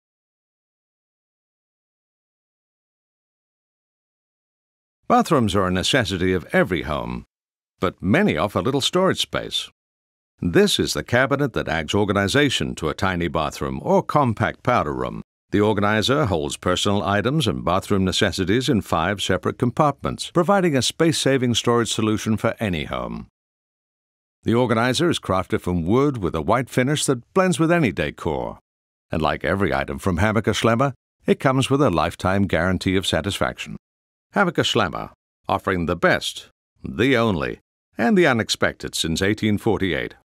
The Tight Space Bathroom Organizer
Available only from Hammacher Schlemmer, this is the cabinet with a narrow footprint but large capacity that adds organization and storage to a tight bathroom space. Ideal for snug powder rooms or storage-starved washrooms with only a pedestal sink, the 9"-wide organizer slips beside a toilet or tub to unobtrusively hold personal items and cleaning products in five separate compartments. The bottom drawer rolls out to reveal a tall cubby for stacks of toilet tissue or a plunger. Two shelves behind this fit spray bottles and soap, while an upper drawer stows cosmetics, razors, and other small articles. The cabinet top conceals a nook for medication and other essentials that need to be accessed easily yet hidden from view. With a clean white finish that blends with any décor, the clever organizer is crafted from wood and rolls on four casters. With external magazine rack. Includes nickel tone and black hardware.